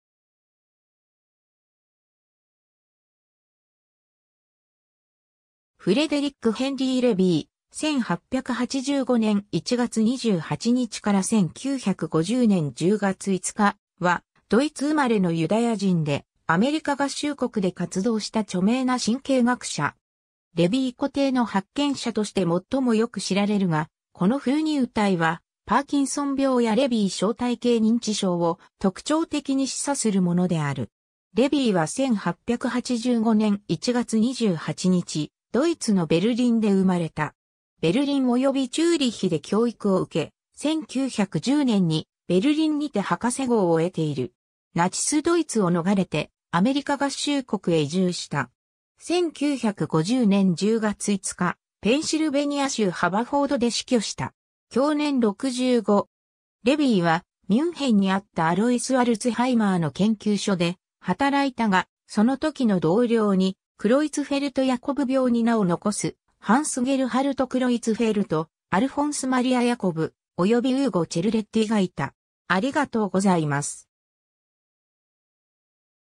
フレデリック・レビー
ナチス・ドイツから逃れたユダヤ人移民
フレデリック・ヘンリー・レビー（Frederic Henry Lewey, 出生時名 フリードリヒ・ハインリヒ・レヴィ（Friedrich Heinrich Lewy）, 1885年1月28日 - 1950年10月5日）は、ドイツ生まれのユダヤ人で、アメリカ合衆国で活動した著名な神経学者。
 レビー小体の発見者として最もよく知られるが、この封入体はパーキンソン病やレビー小体型認知症を特徴的に示唆するものである。
 レビーは1885年１月28日、ドイツのベルリンで生まれた。
ベルリンおよびチューリッヒで教育を受け、1910年にベルリンにて博士号を得ている。
ナチス・ドイツを逃れてアメリカ合衆国へ移住した。
1950年10月5日、ペンシルベニア州ハバフォードで死去した。
享年65。
 レビーはミュンヘンにあったアロイス・アルツハイマーの研究所で働いたが、その時の同僚にクロイツフェルト・ヤコブ病に名を残すハンス・ゲルハルト・クロイツフェルト（1885年 - 1964年）、アルフォンス・マリア・ヤコブ（1884年 - 1931年）、およびウーゴ・チェルレッティ（1877年 - 1963年）がいた。